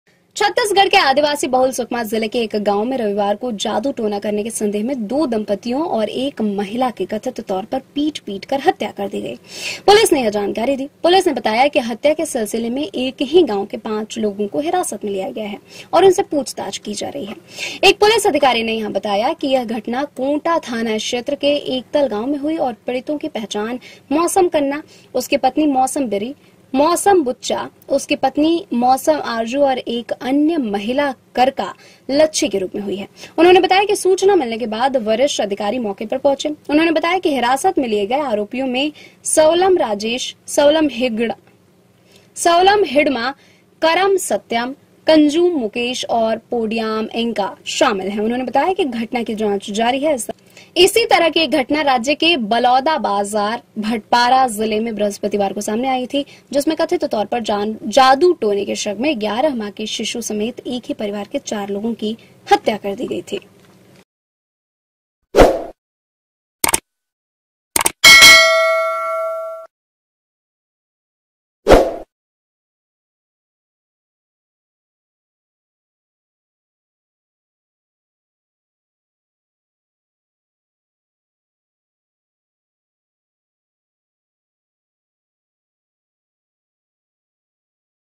Chhattisgarh : जादू-टोने का शक, 3 महिलाओं समेत 5 की हत्या, 5 लोगों को लिया हिरासत में
छत्तीसगढ़ के आदिवासी बहुल सुकमा जिले के एक गांव में रविवार को जादू-टोना करने के संदेह में 2 दंपतियों और एक महिला की कथित तौर पर पीट-पीटकर हत्या कर दी गई। पुलिस ने यह जानकारी दी। पुलिस ने बताया कि हत्या के सिलसिले में एक ही गांव के पांच लोगों को हिरासत में लिया गया है और उनसे पूछताछ की जा रही है।